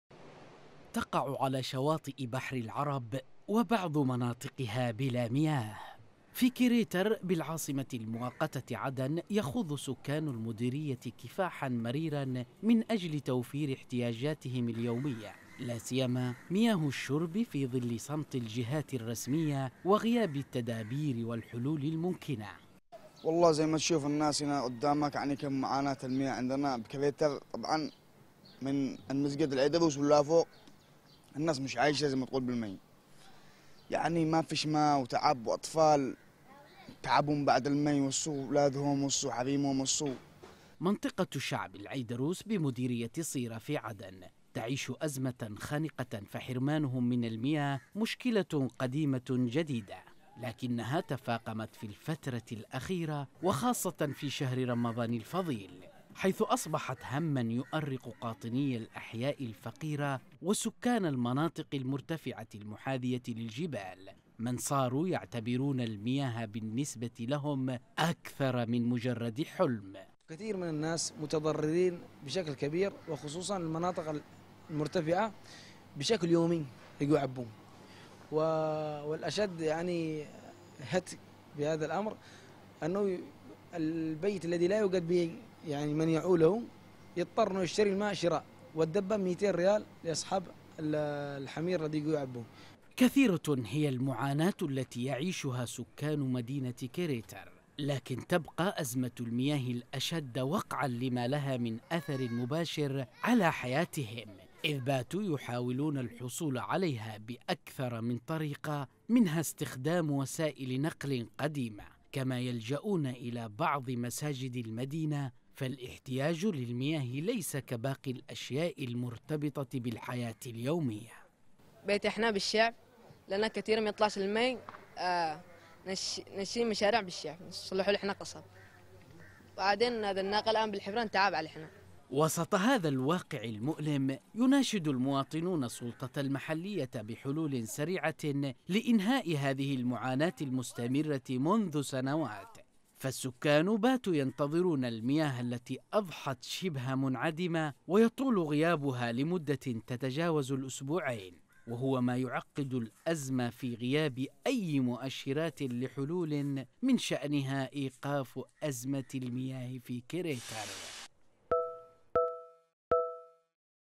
عدن..أزمة مياه خانقة في منطقة شعب العيدروس ومناشدات بإيجاد الحلول | تقرير: أديب الجناني
يعاني سكان منطقة شعب العيدروس بمديرية صيرة في العاصمة المؤقتة عدن في الحصول على مياه نقية.
ويشكو المواطنون في المنطقة من تفاقم هذه الأزمة خاصة خلال الفترة الأخيرة.
وناشد المواطنون السلطة المحلية بإيجاد حلول سريعة لإنهاء هذه المعاناة المستمرة منذ سنوات...